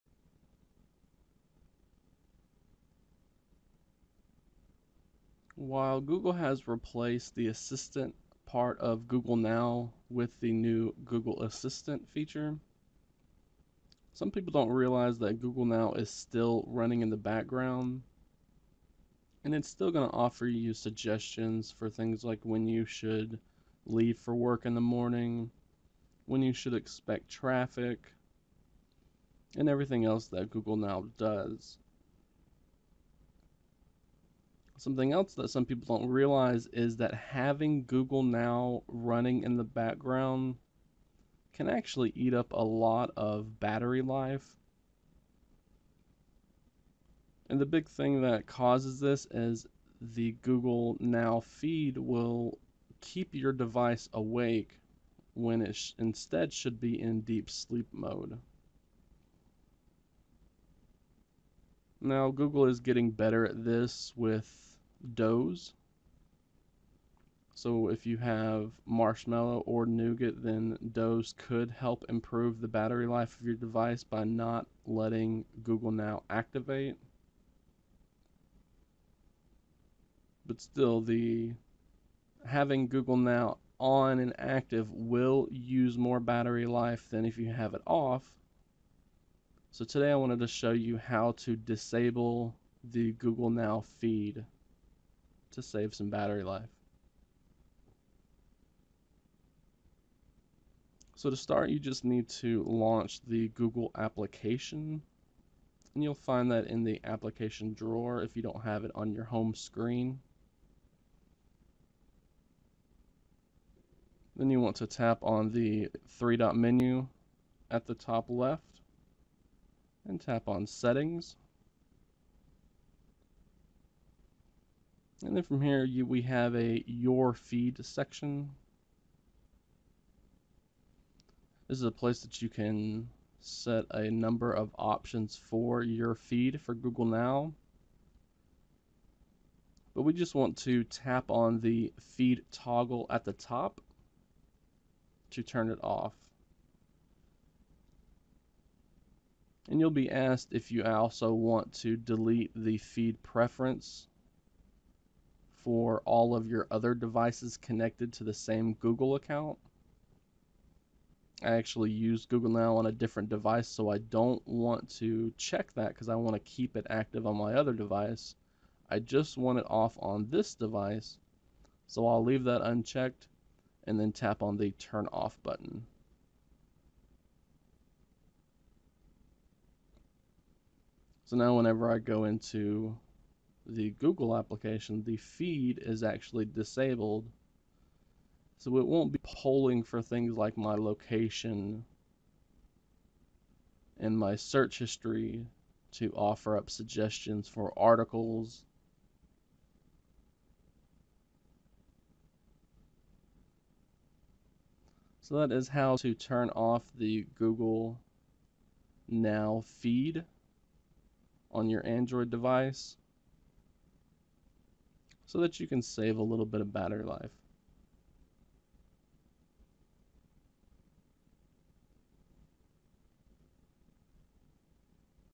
Turning off the Google Now Feed
If you want to save some battery life, or just don't need the feature, then you can follow these steps to turn off the Google Now Feed.